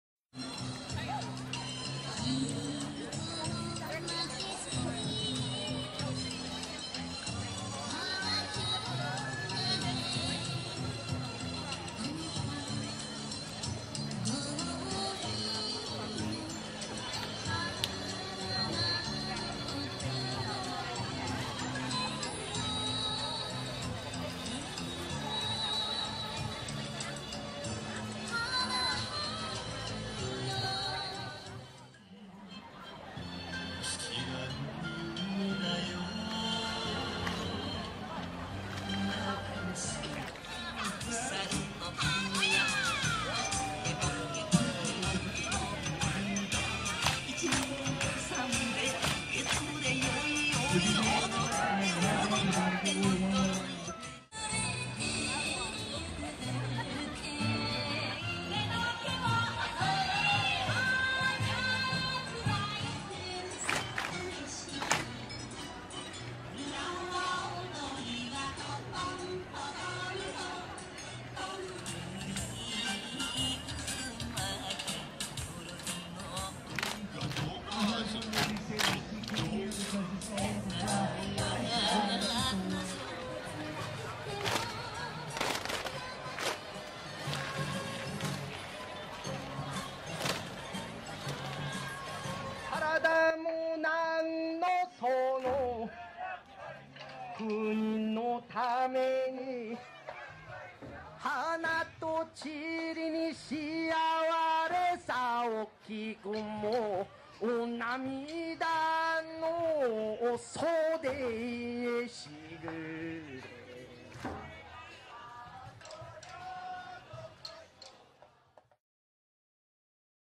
2013 Moiliili Bon Dance
My impressions of the annual Moiliili Hongwanji bon dance.